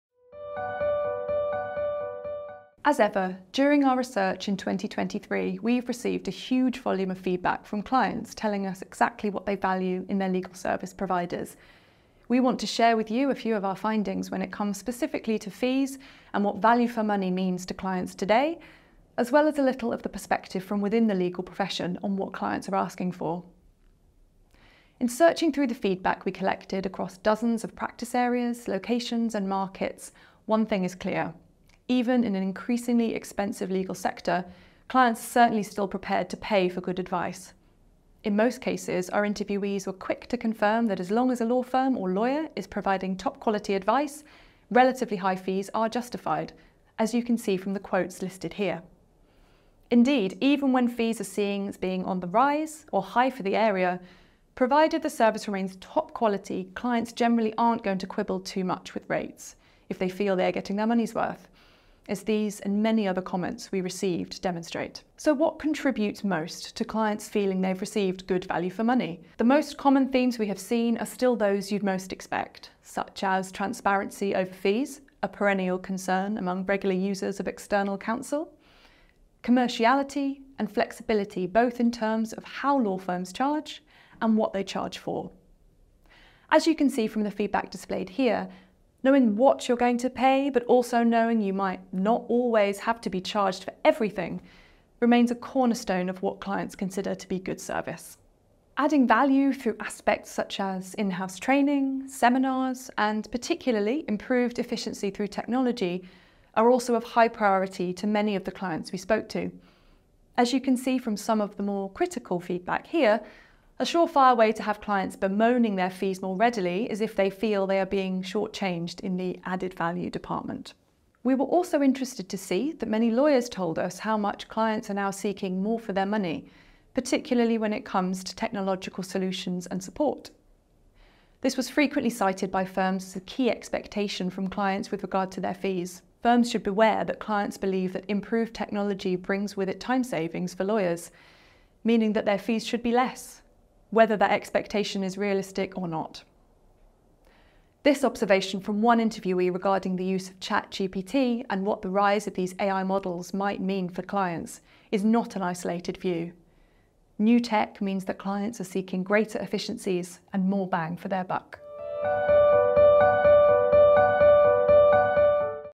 What do Clients of Legal Services Value the Most? | Chambers and Partners
In this video, the Chambers UK team shares a few of our findings when it comes specifically to fees and what ‘value for money’ means to clients today, as well as a little of the perspective from within the legal profession on what clients are asking for.